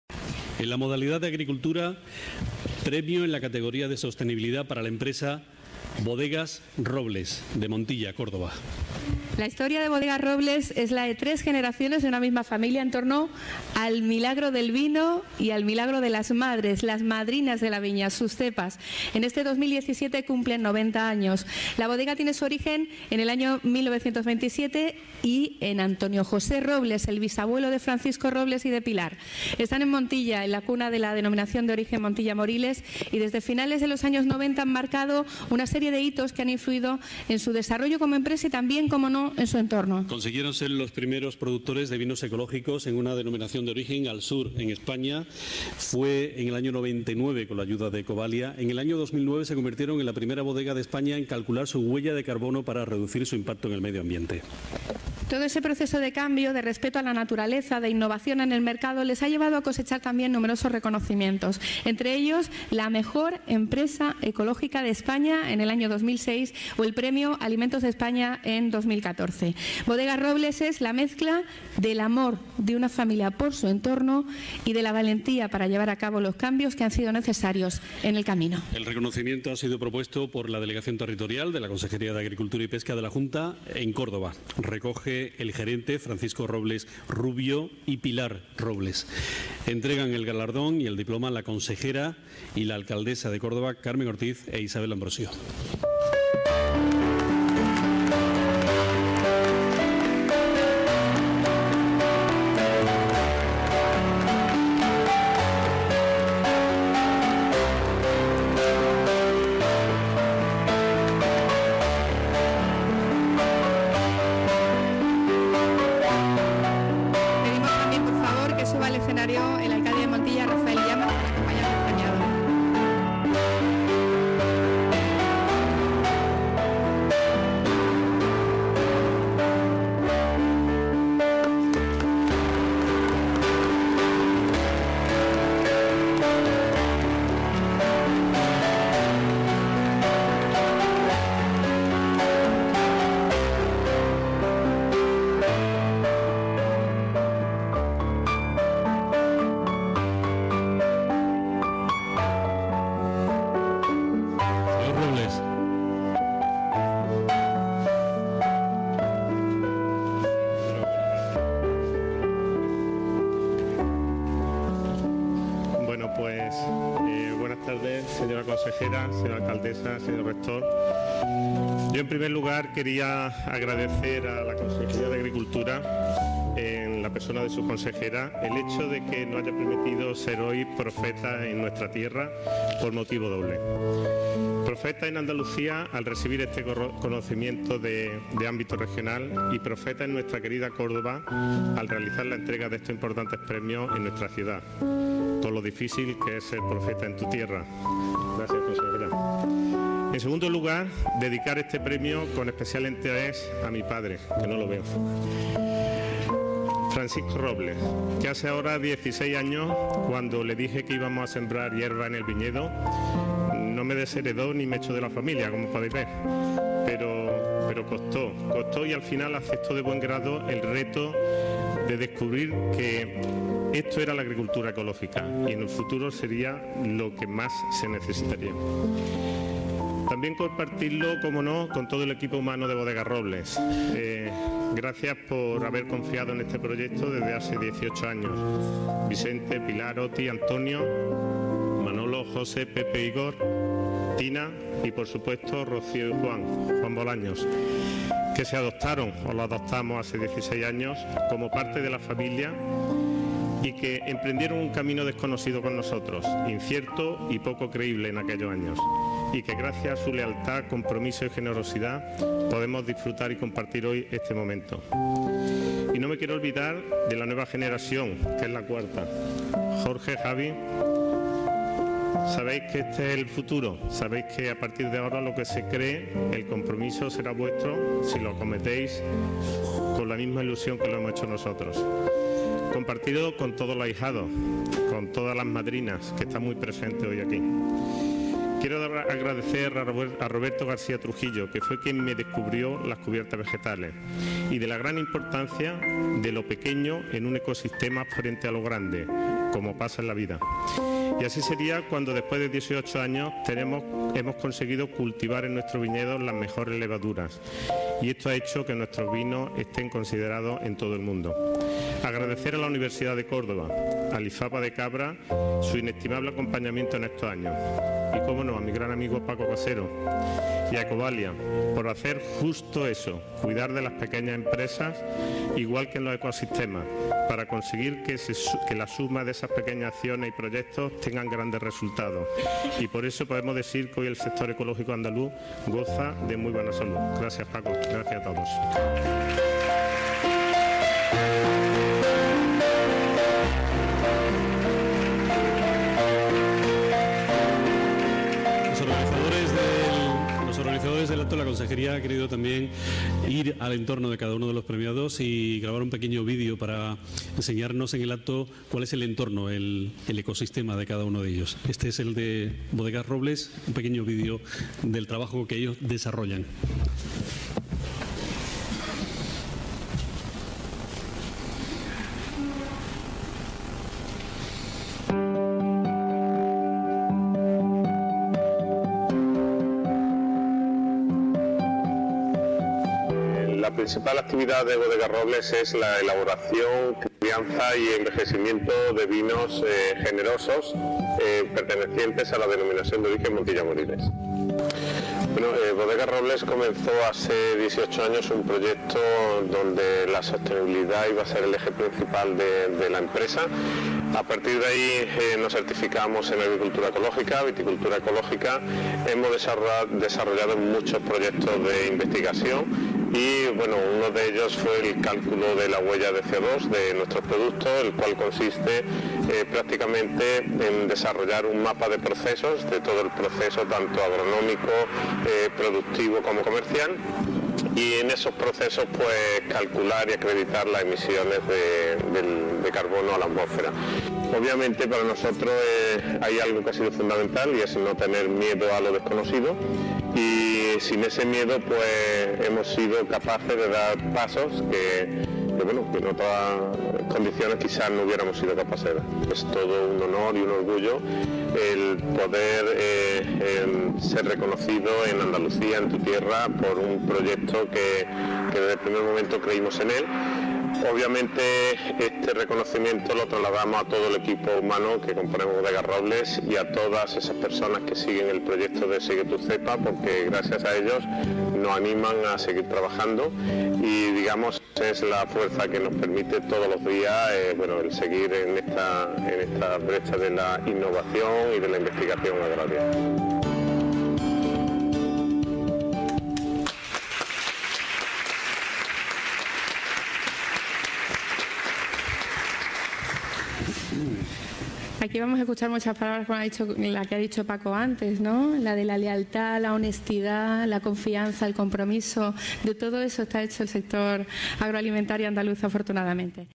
Bodegas Robles en los Premios Andalucía de Agricultura y Pesca.
Pilar Robles y Francisco Robles recogieron el Premio a la Sostenibilidad otorgado a Bodegas Robles en los Premios de Andalucía de Agricultura y Pesca que organiza anualmente la Junta de Andalucía y que se celebraron el pasado 16 de mayo de 2017 en el Rectorado de la Universidad de Córdoba.